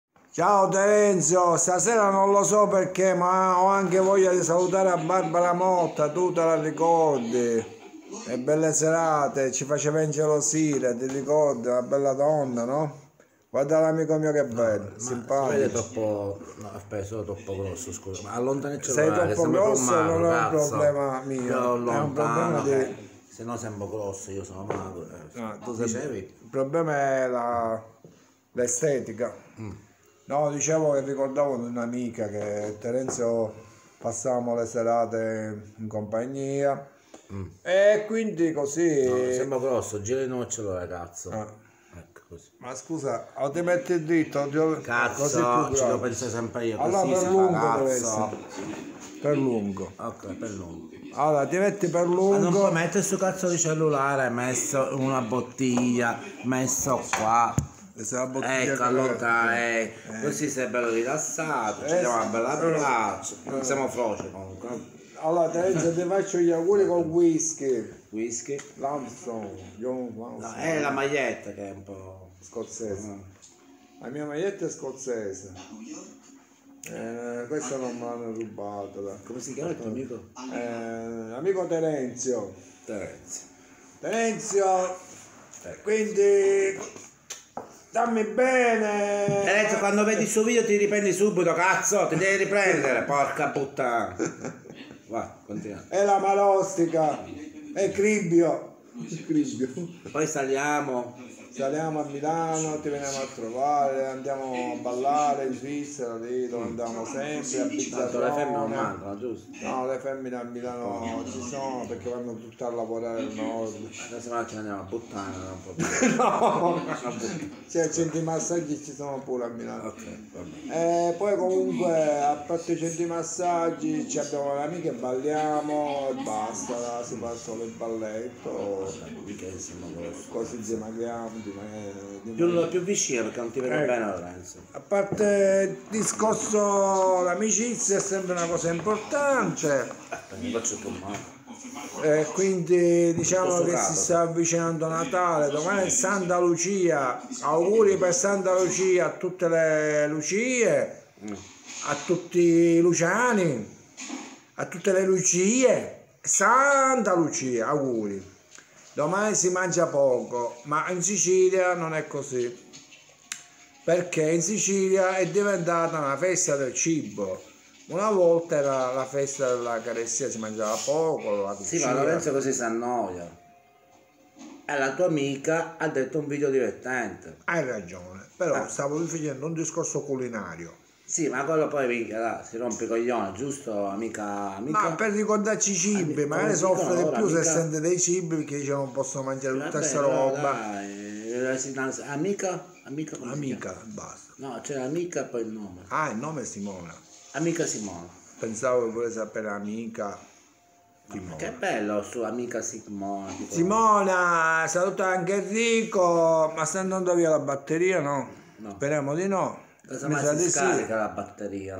Massimo ed io Saluti A Milano Simona E Terenzio
dopo una giornata di lavoro in diretta da Palermo con Simona in Milano Saluti a Terenzio